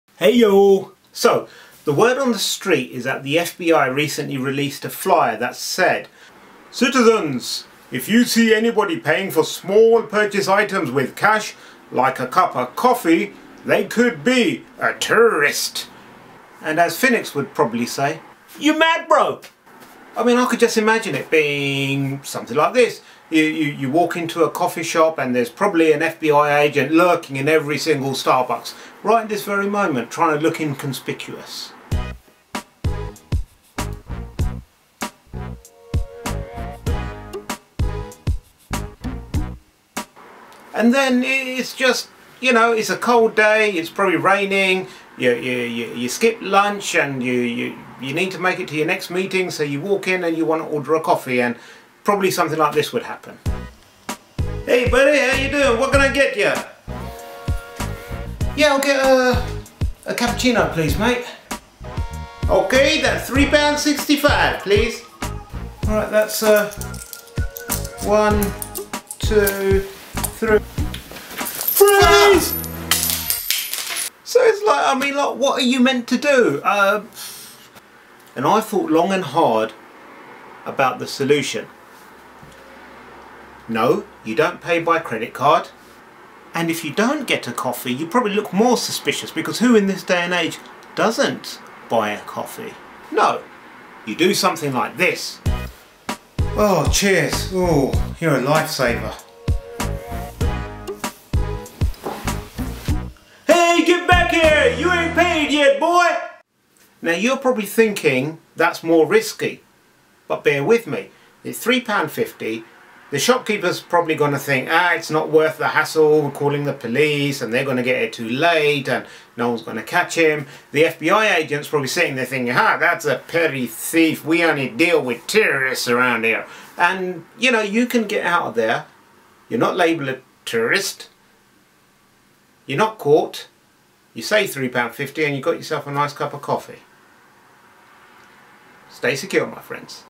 FBI Coffee
The FBI recently stated that people who pay in cash for small items such as coffee or chewing gum are probably dubious individuals who are trying to blend in... whats a guy to do?

Facebook.com/J4vv4D
Twitter.com/J4vv4D